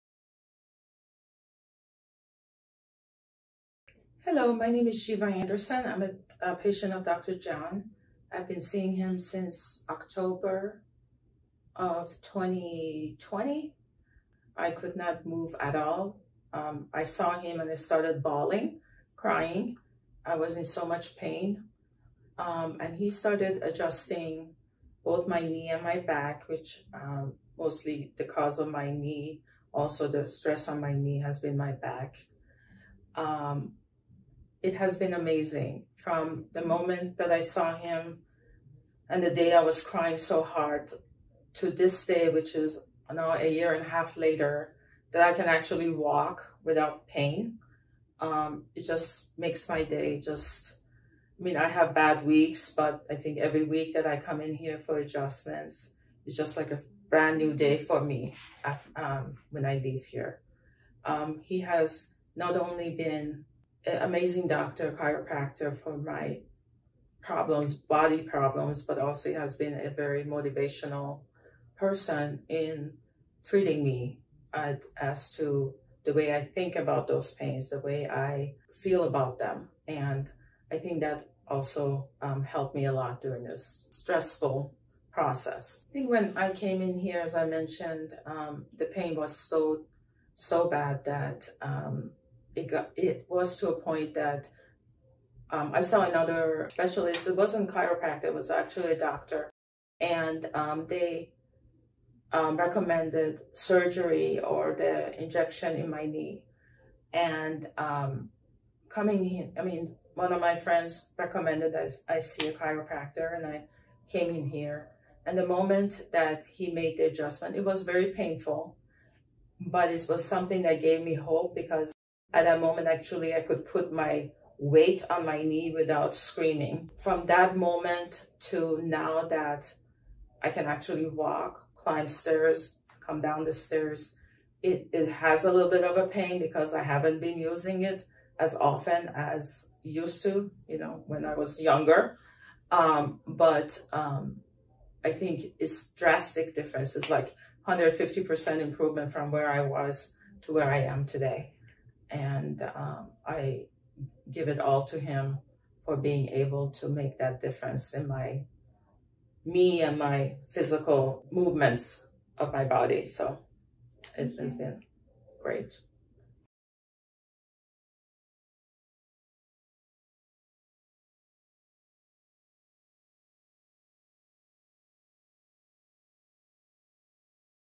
Severe Knee & Back Pain Eased Through Specific Chiropractic Care
Before chiropractic care with Dr. John, Shiva had such severe knee pain that she was having trouble walking and doing day-to-day tasks. She was also suffering from back pain. When she came to her initial appointment, she was crying from her pain. Since coming to Cornerstone Family Chiropractic in October of 2020, Shiva has been successfully guided towards recovery. Now she can walk much better and says there is a 150% improvement/turnaround in how she feels and views her body and pain. She is very grateful for Cornerstone Family Chiropractic.

Watch the entire video to learn more about Shiva’s successful Specific Chiropractic Care journey.

Shiva’s case is a wonderful piece of proof to the effectiveness of receiving Specific Chiropractic Care. We want the same for you.

Watch our videos to discover more about Specific Chiropractic Care.